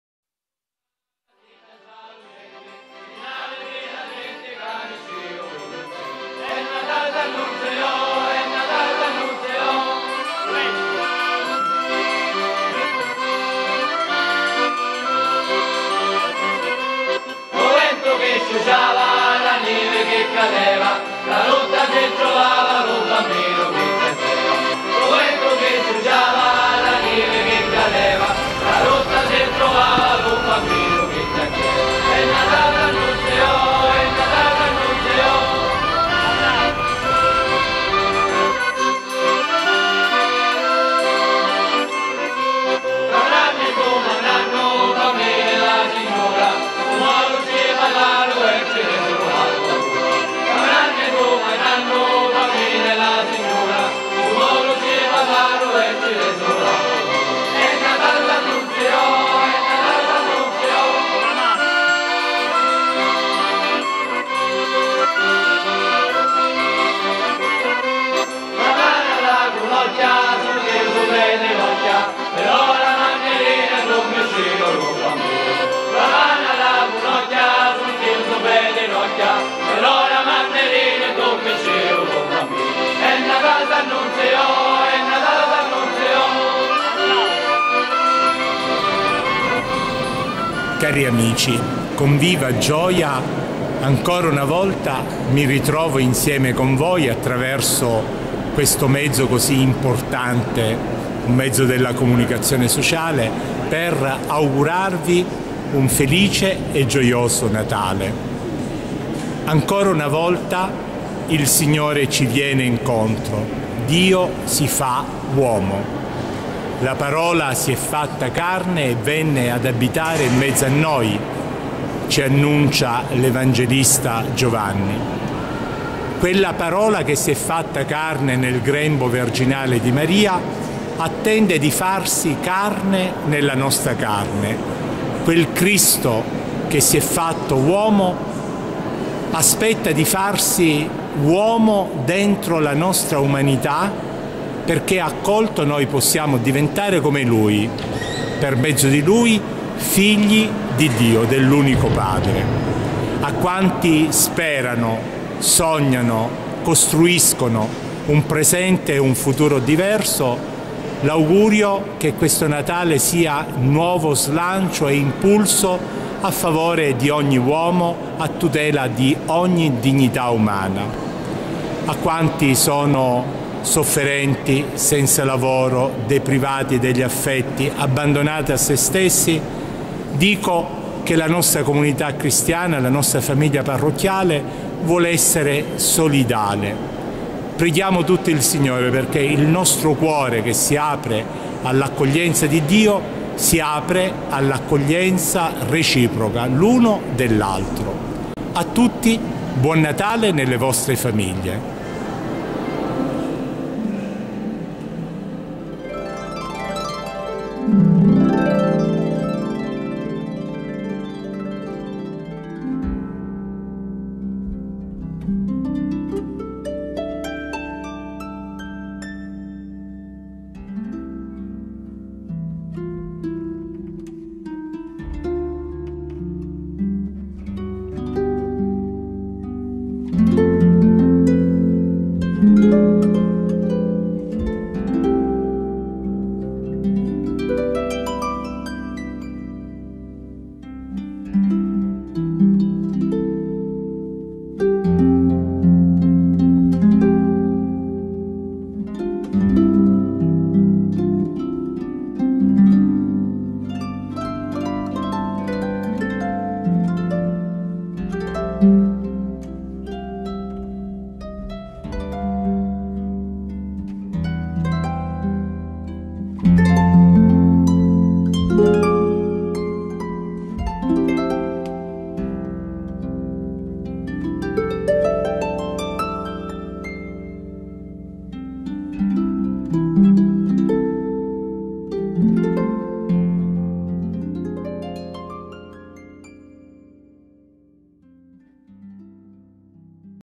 Don Santino Colosi, auguri al web
Il video tratta degli auguri al web di Don Santino Colosi, arciprete del Duomo di Milazzo e di altre scene girate by night a Milazzo